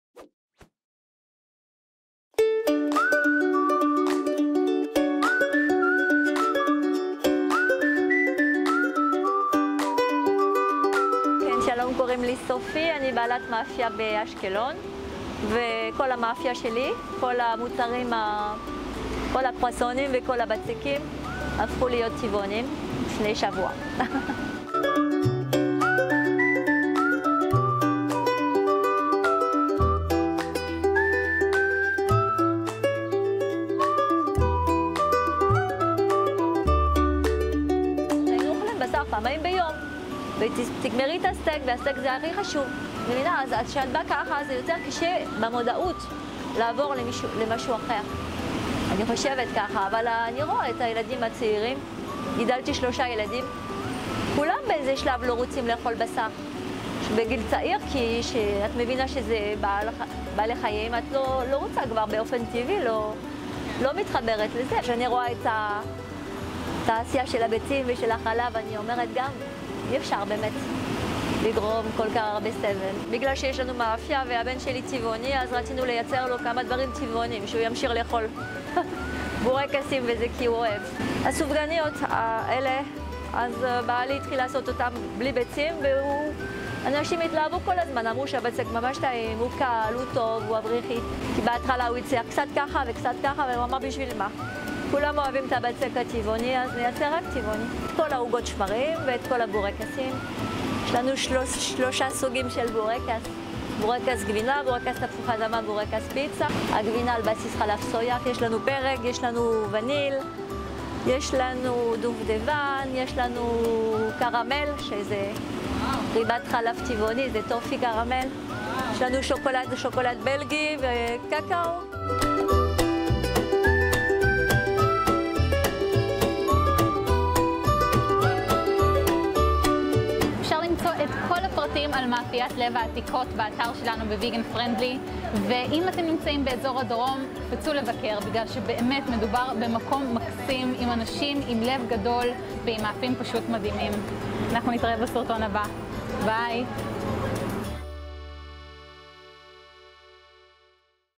סיפור מרגש - מאפייה שקיימת כבר 23 שנים הפכה לטבעונית!
מוזמנים להכיר את סופי ושמעיה, בעלי מאפיית "לב העתיקות" באשקלון. המאפייה קיימת כבר 23 שנה ולאחרונה הם החליטו להפוך כמעט את כל המאפים שלהם לטבעוניים! אפשר למצוא אצלהם מגוון עצום של מאפים ועוגות טבעוניות שנעשו באהבה גדולה על ידי אנשים עם לב ענק. המאפייה נמצאת ברח' ההסתדרות 40, קניון לב, אשקלון, ואנחנו בטוח נחזור לבקר!